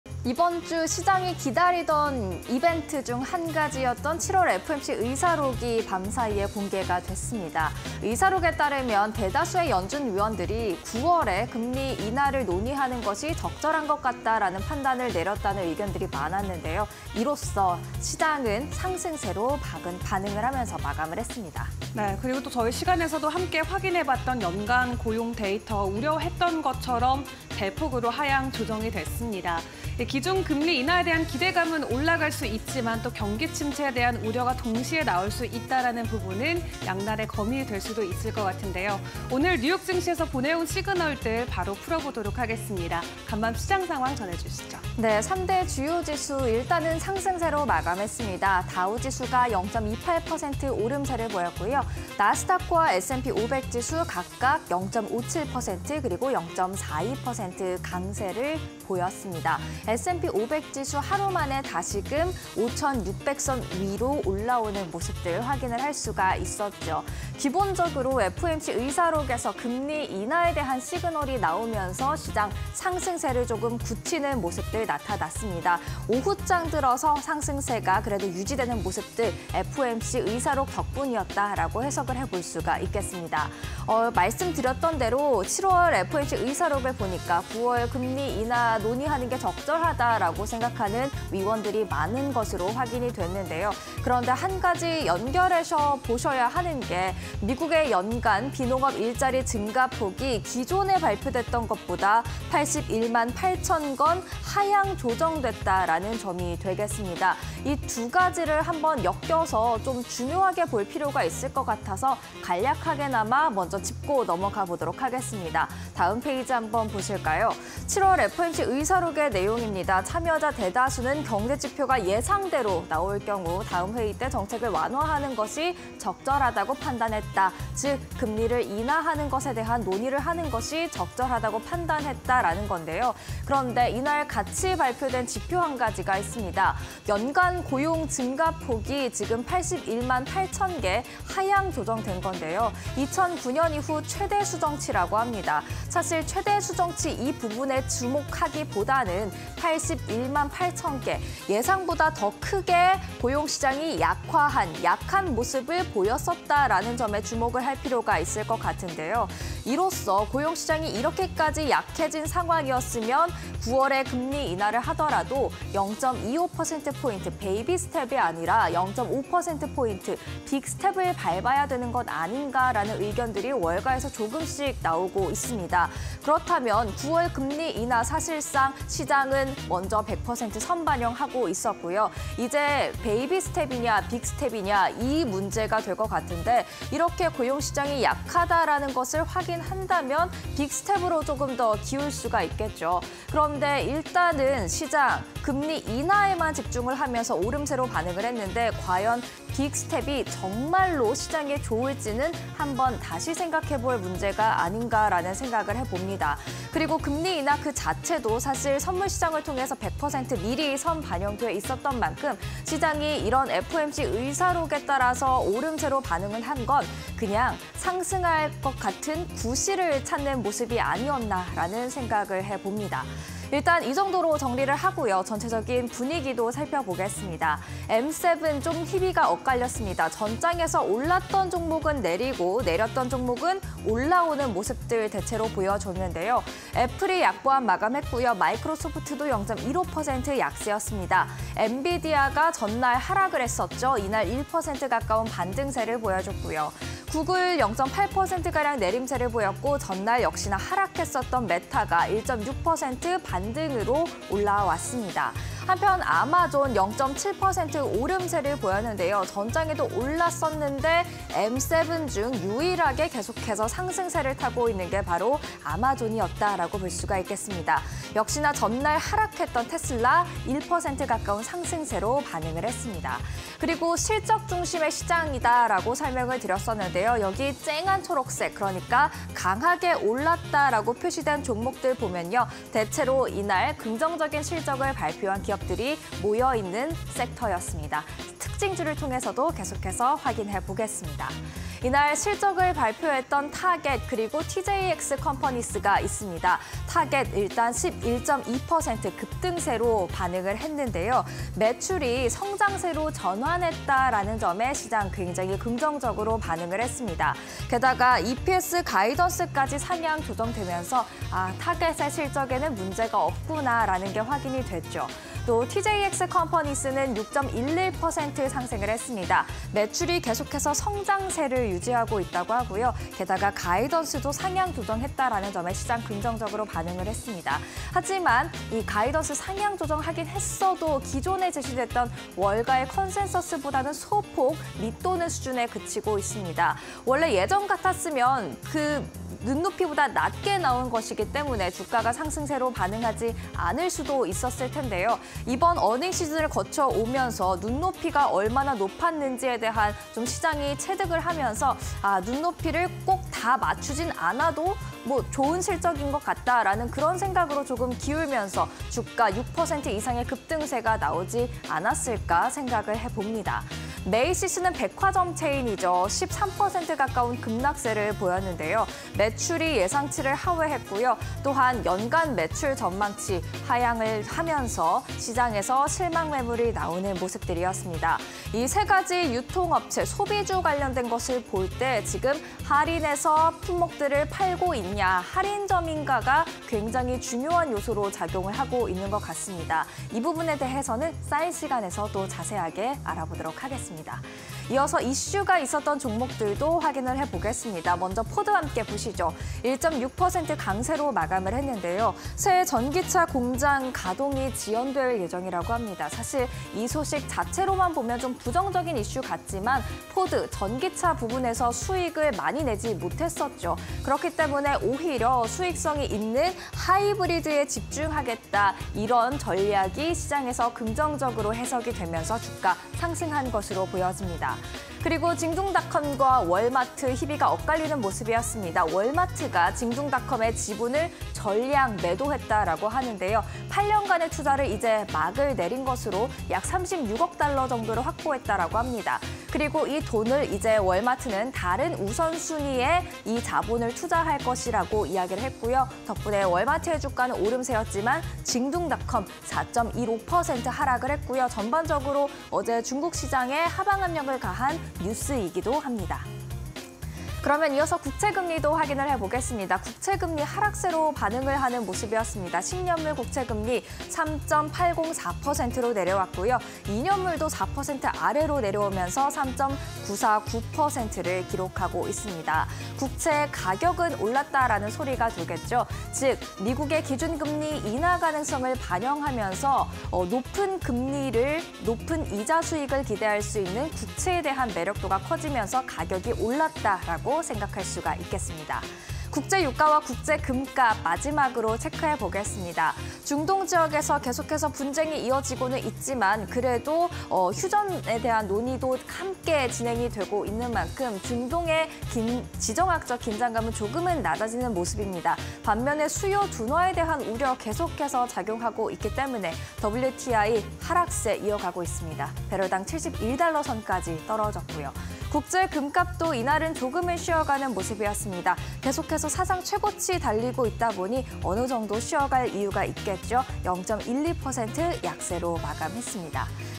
🚀 9월 금리인하... '킥' 시그널 나왔습니다 | Market Signal (20240822)
▶ 뉴욕증시, ‘9월 금리인하’ 가능성에 상승세
▶ M7, 희비 엇갈려... 실적 중심 시장
▶ 타겟: 매출, 성장세로 전환... EPS 가이던스 상향 조정
▶ TJX 컴퍼니스: 매출 성장세 유지... 가이던스 상향 조정
▶ 메이시스: 매출 예상치 하회... 연간 매출 전망 하향
▶ 국제 유가, 미 고용 둔화에 하락 속도↑... $72선
▶ 국제 금값, FOMC 의사록 확인 뒤 상승세 굳혀